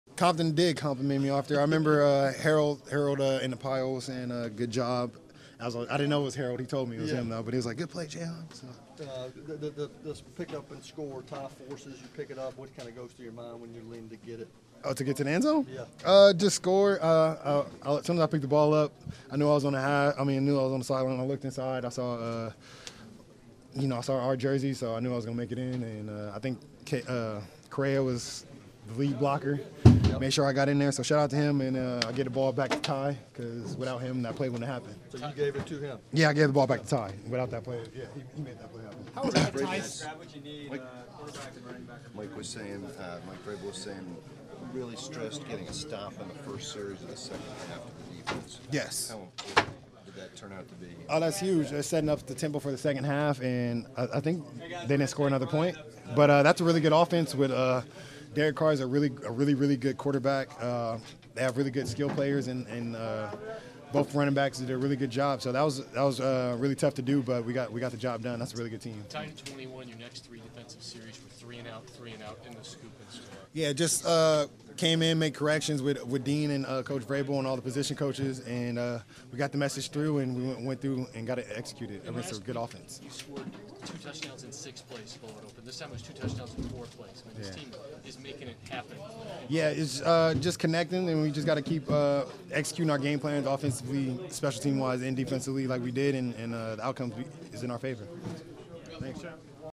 Jayon Brown: That's a Really Good Offense, But We Got the Job Done
Titans LB Jayon Brown speaks with reporters following Tennessee’s 42-21 victory over the Oakland Raiders. The Titans return home to face the Houston Texans for the AFC South lead on Sunday, Dec. 15 at Nissan Stadium.